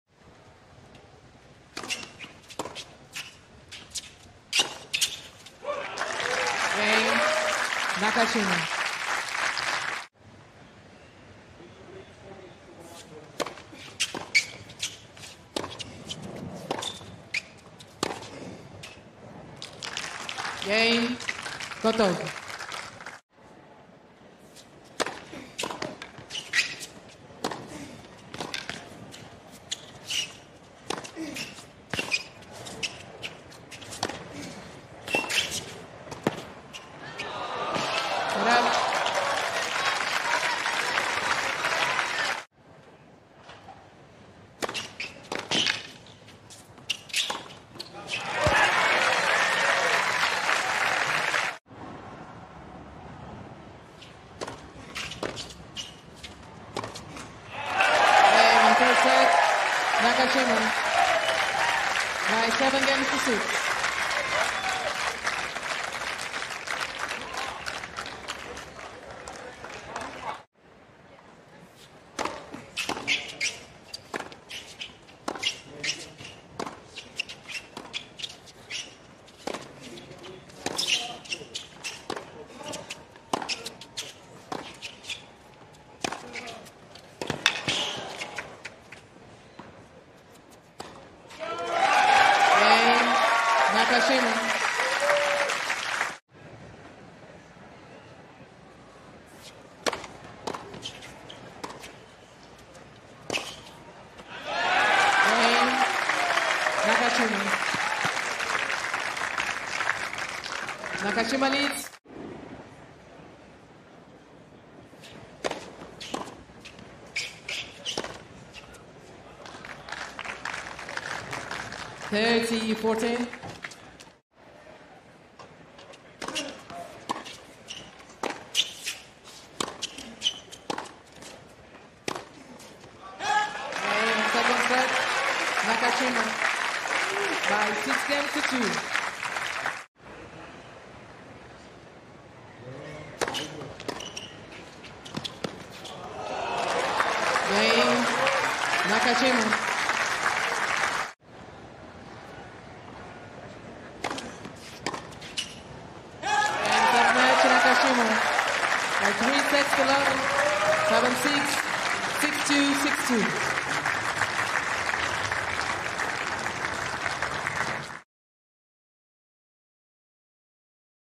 Brandon Nakashima vs. Pavel Kotov Highlights | 2022 US Open Round 1
Watch the highlights between Brandon Nakashima and Pavel Kotov in Round 1 of the 2022 US Open.

These highlights are presented by Cadillac.